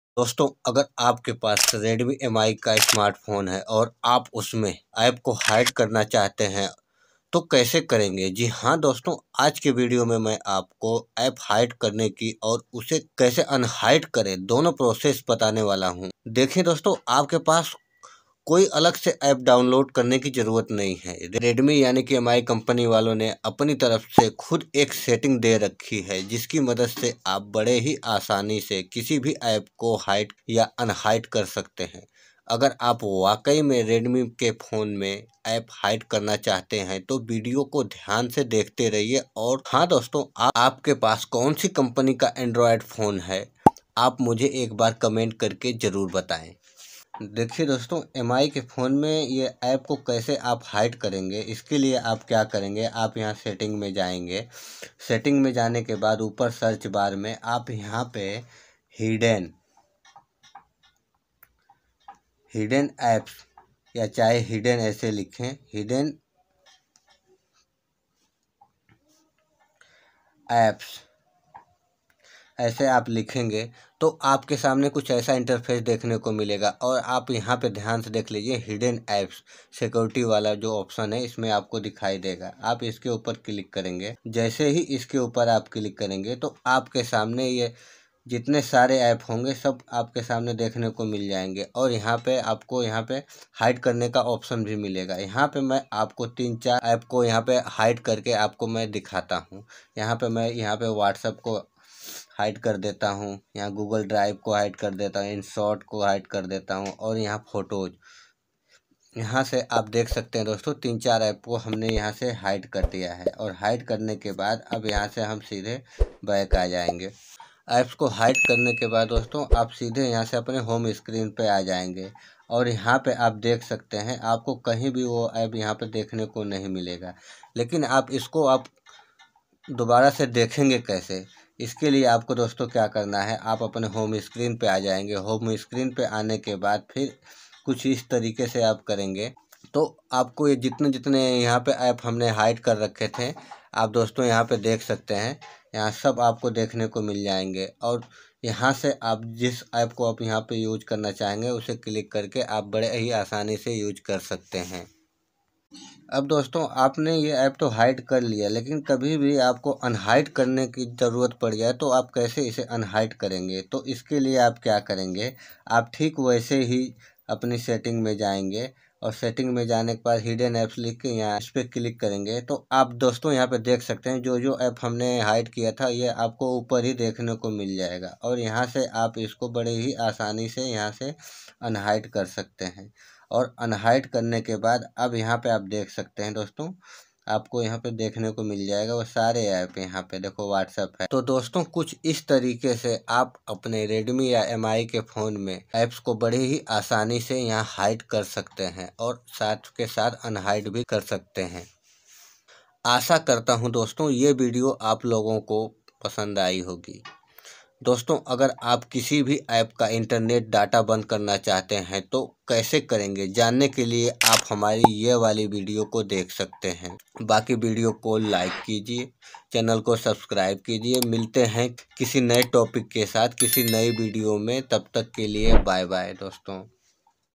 Redmi Phone Me App Hide Kaise Kare | How To Hide App In Redmi Mobile | Hide Apps In Mi Redmi Phone
Hallo dosto ke video me aap janenge Redmi Phone Me App Hide Kaise Kare, How To Hide App In Redmi Mobile is video me aapko poori jankari one by one kar ke milne wali hai. isliye video ko poora jaroor dekhe our sath me video Ko like our subscribe kar ke support kare.